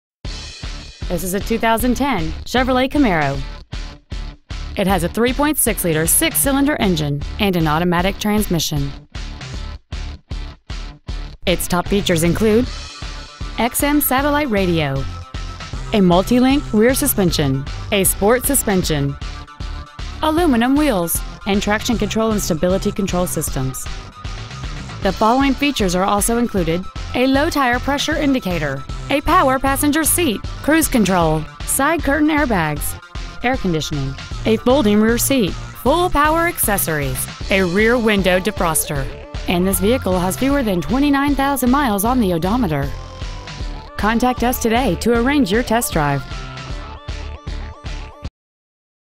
2010 Chevrolet Camaro Ivel KY
We are proud to present this 2010 Chevrolet Camaro . We have been honored to serve the Ivel KY area , we promise that your experience at our dealership will exceed your expectations ! Year : 2010 Make : Chevrolet Model : Camaro Engine : 3.6L V6 24V GDI DOHC Trans .